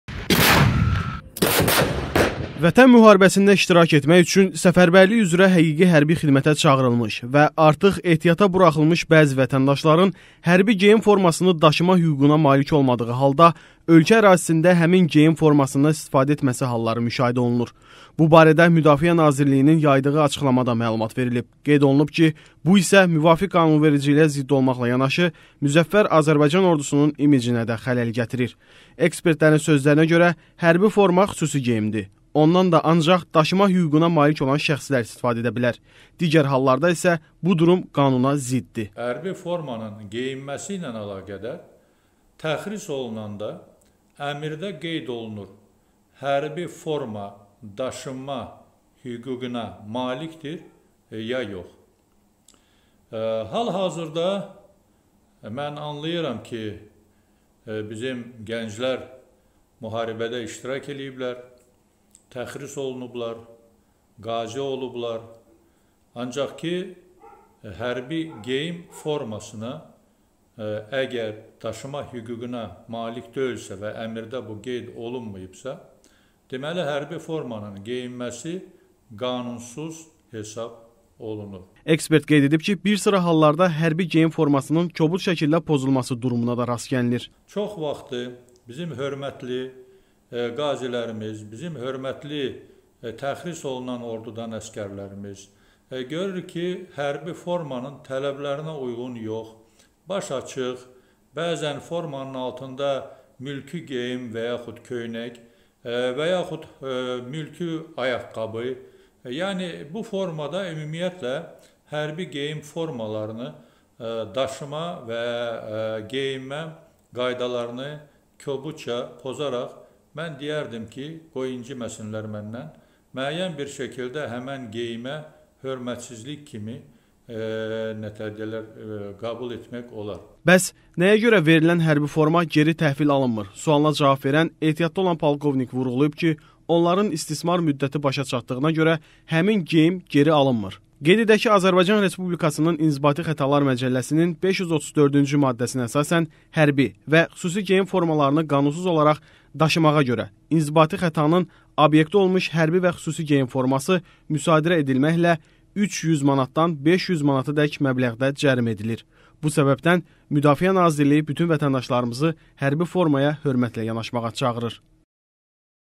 Pozulan hərbi geyim qaydaları
Müdafiə Nazirliyi vətəndaşları hərbi formaya hörmətlə yanaşmağa çağırır